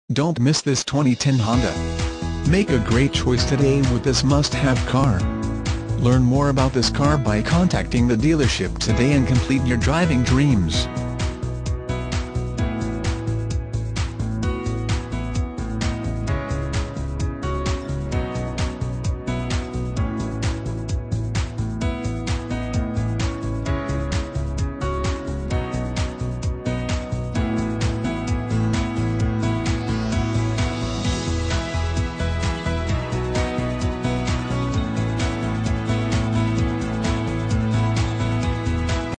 2010 HONDA ACCORD CPE Littleton, CO
2010 HONDA ACCORD CPE Littleton, CO
Stock #01703

You'll love this 2010 Honda Accord Cpe. This is a car you'll want to take home. With 6 miles, it features manual transmission and an exterior color of Alabaster Silver Metallic. Call us and be the first to open the car door today!